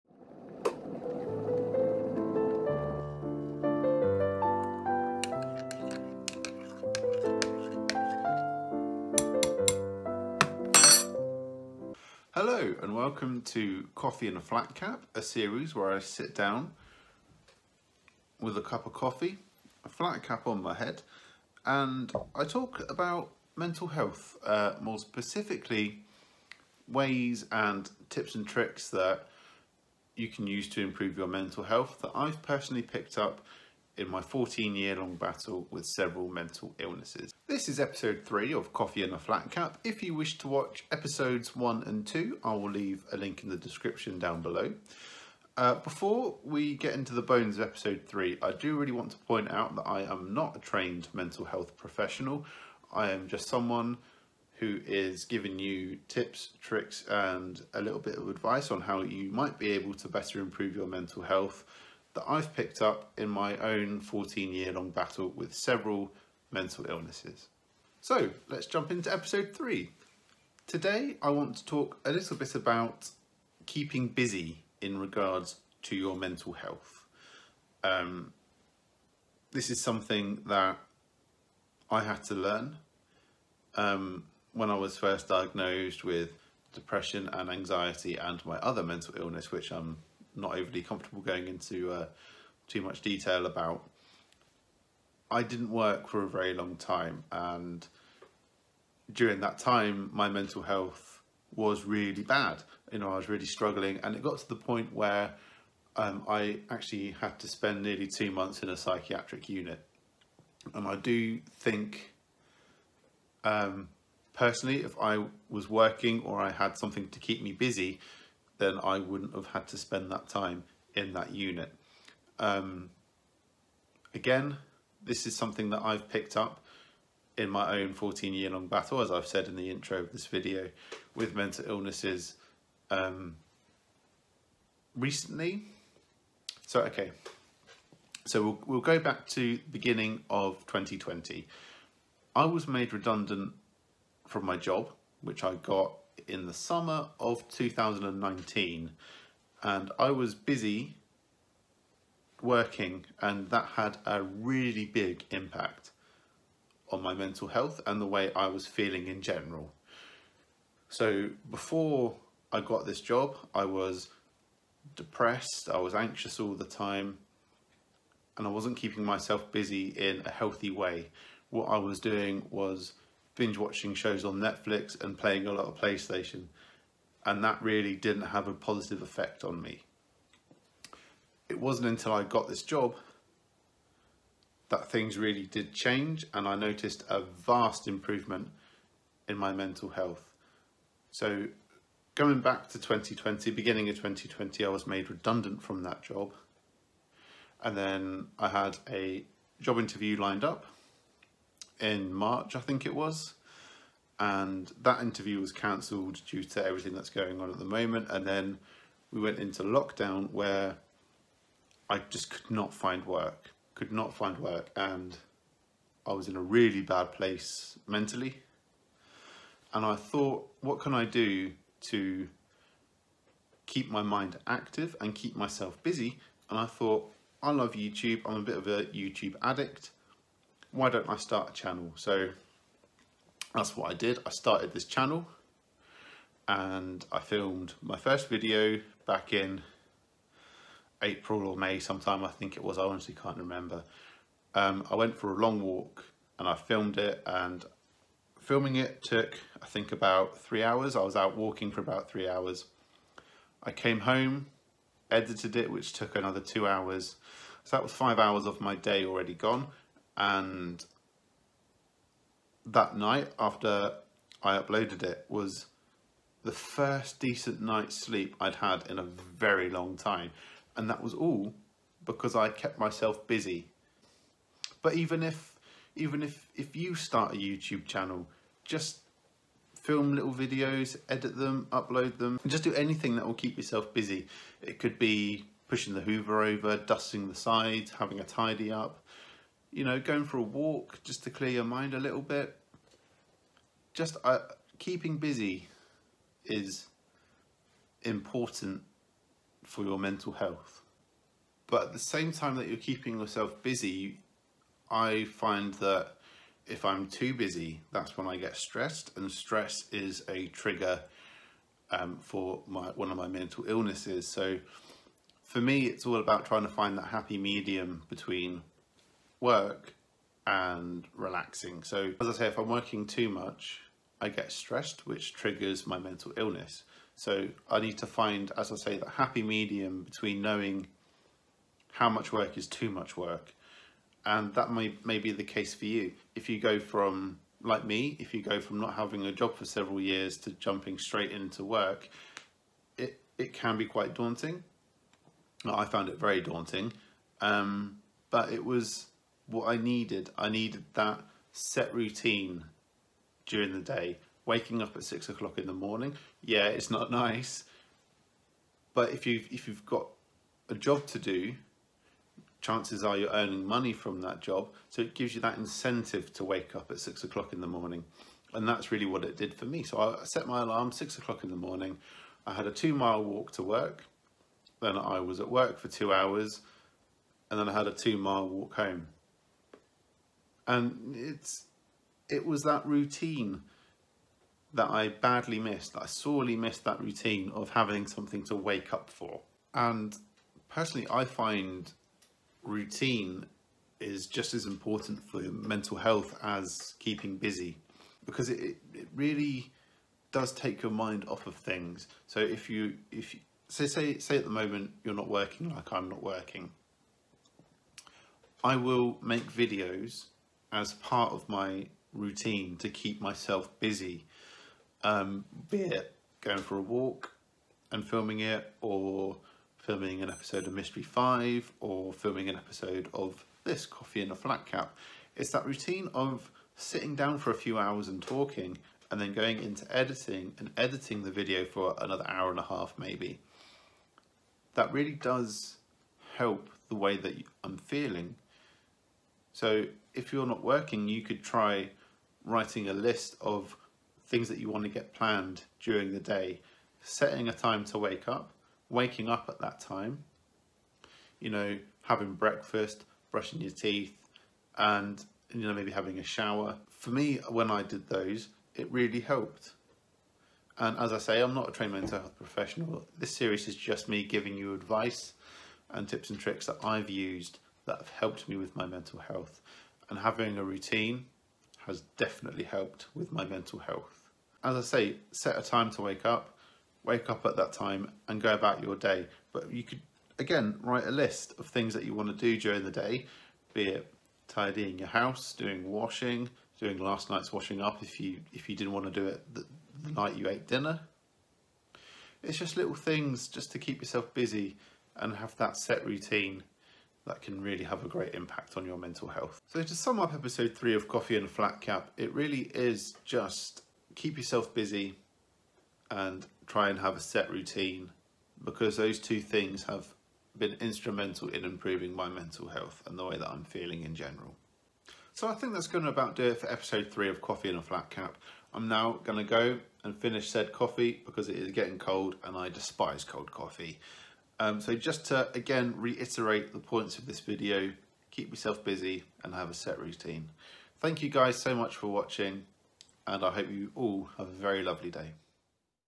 Keeping Busy, Having A Routine And Mental Health | Coffee And A Flat Cap Ep 3
Hey, I hope you're keeping well! In this, episode 3, of Coffee And A Flat Cap I talk about how keeping busy and having a routine definitely helps in regards to improving your mental health.

Thank you for taking the time to watch, I hope this series is helping.